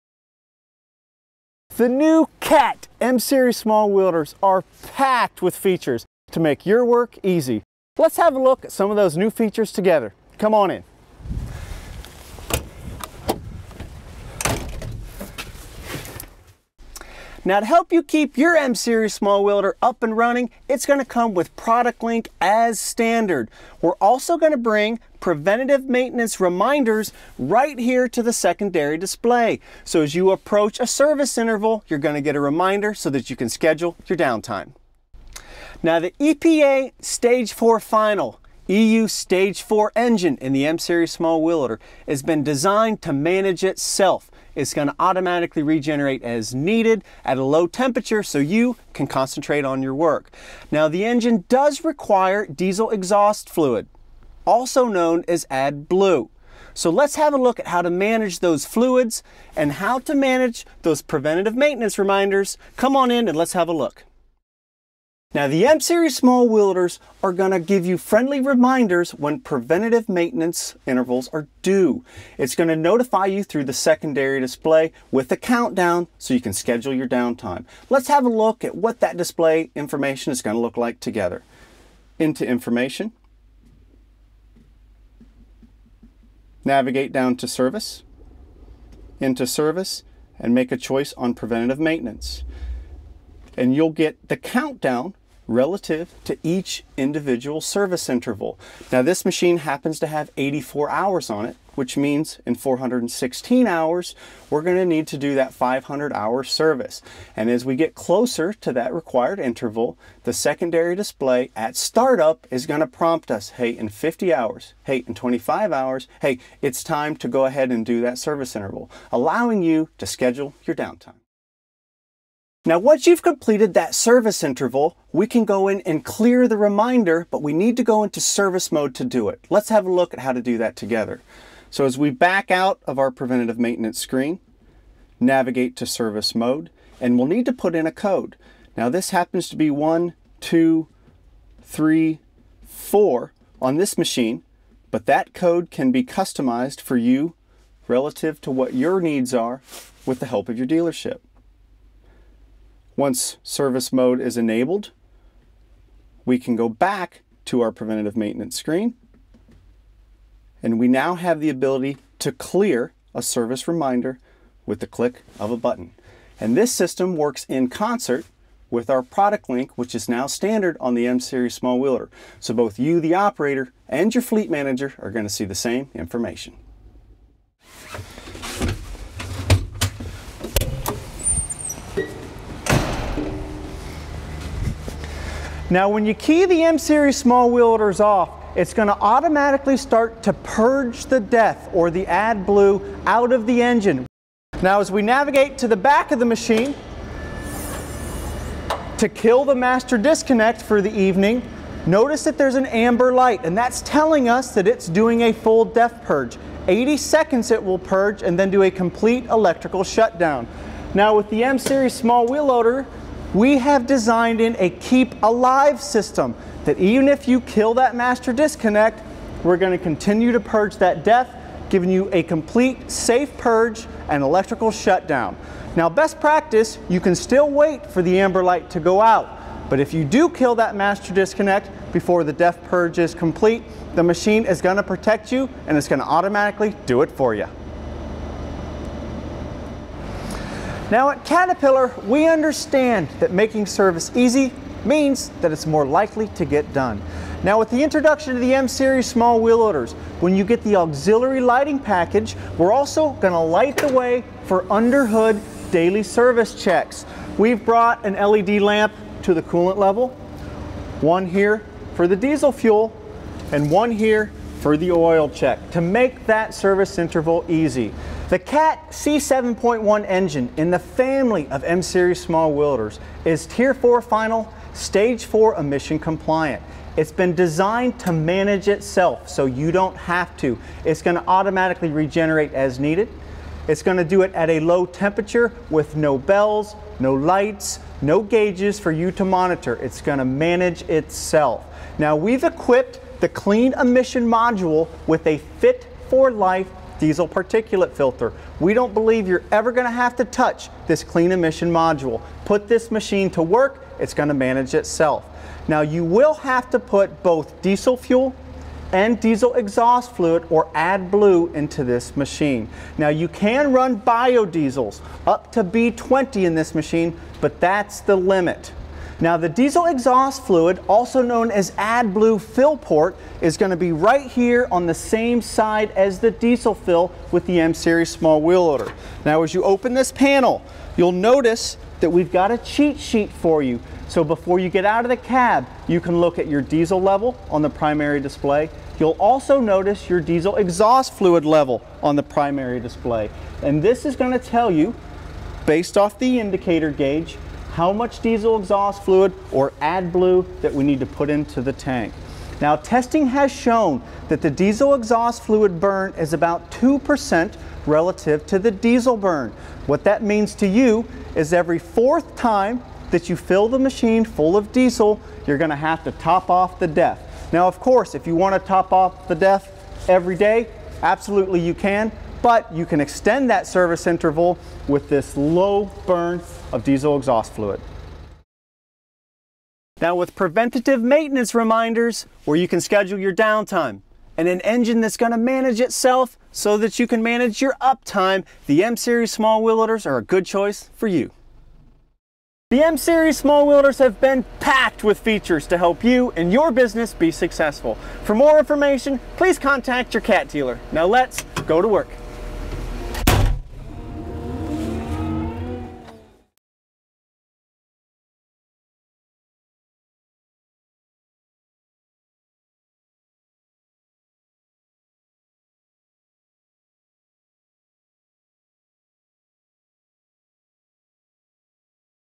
Preventative Maintenance (Cat® 926M, 930M, 938M Wheel Loader Operator Tips)
| Preventative maintenance is an important part to the life of your wheel loader.  The Cat® M Series family will help make your job a little easier by adding key maintenance interval reminders directly into the optional secondary display. Come take a look as Joel Grimes, Caterpillar product expert, reviews the various reminders built into the display as well as the diesel exhaust fluid (DEF) fill interval.